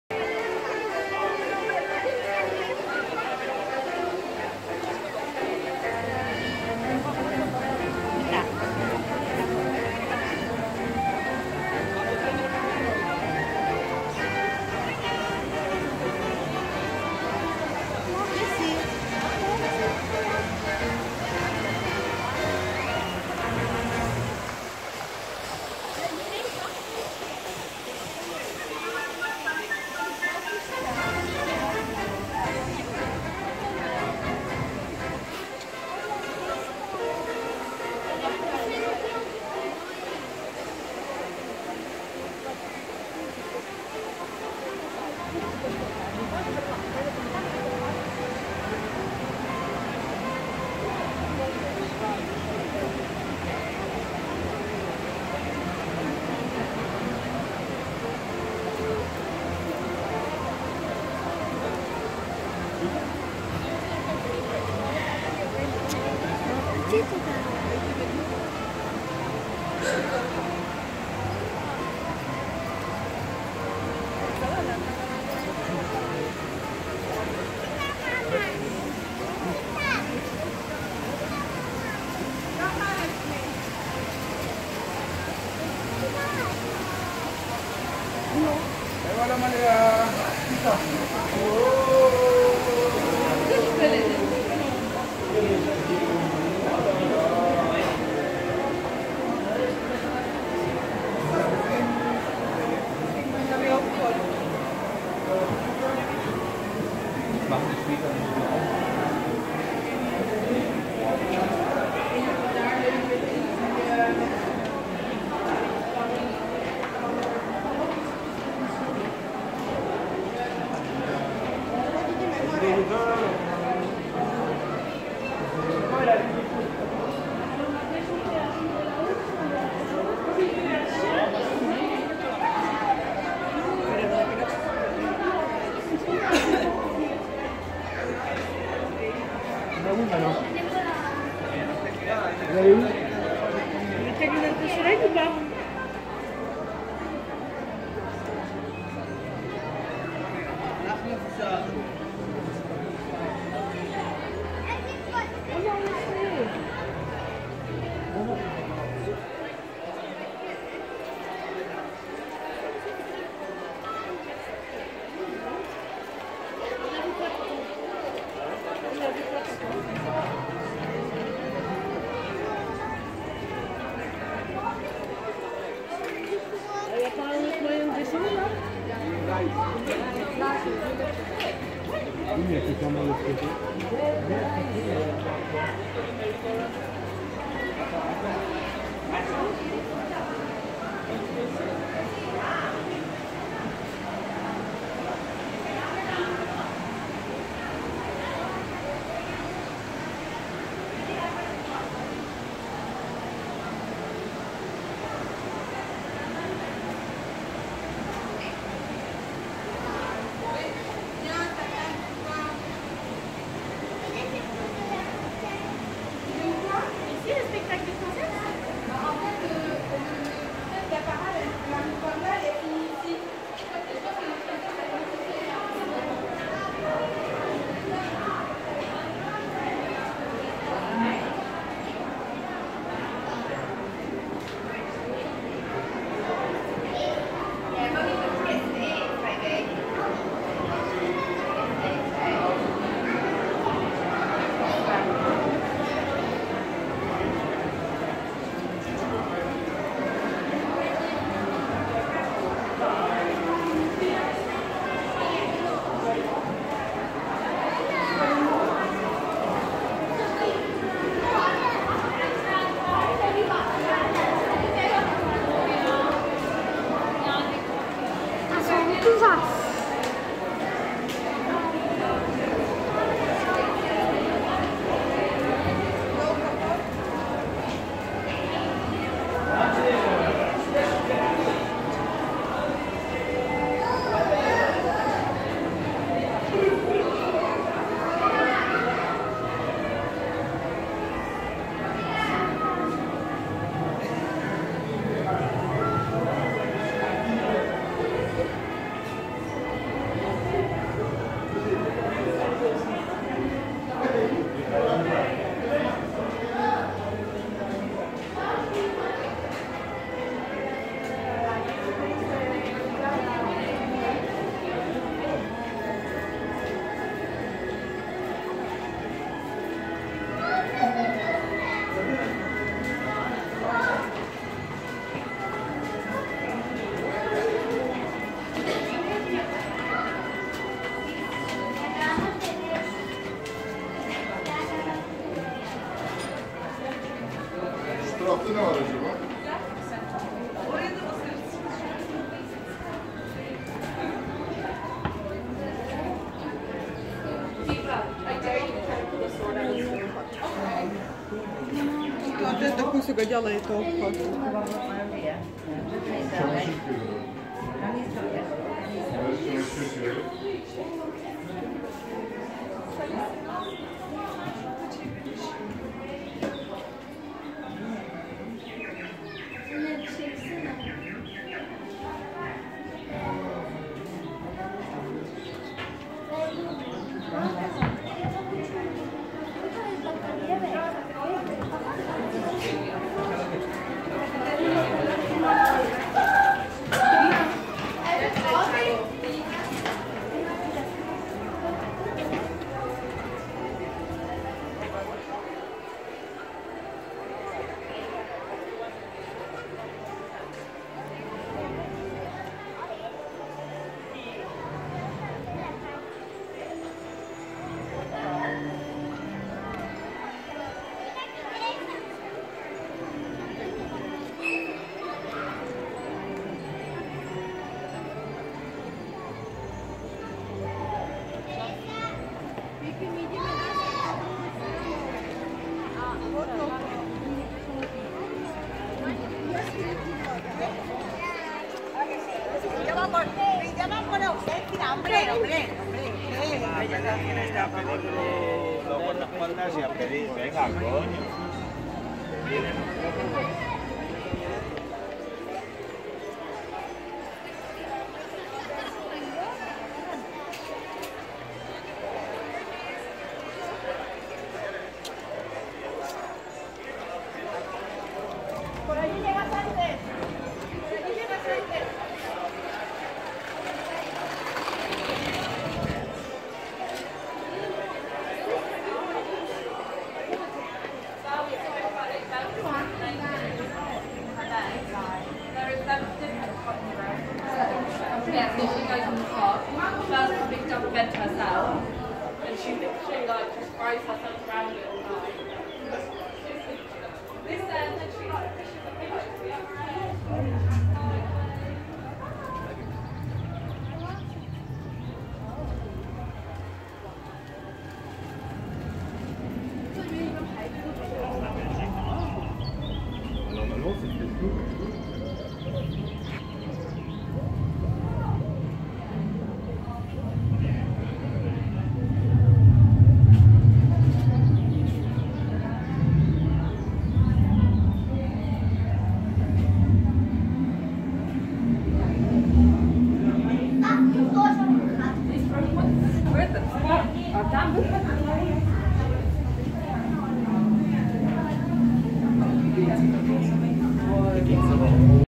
(1080p60) | Vlog Paríž 03 | Disneyland: Fantasyland
Prehliadka parku, konkrétne časť Fantasylandu. Točené mobilom Samsung Galaxy S7 edge.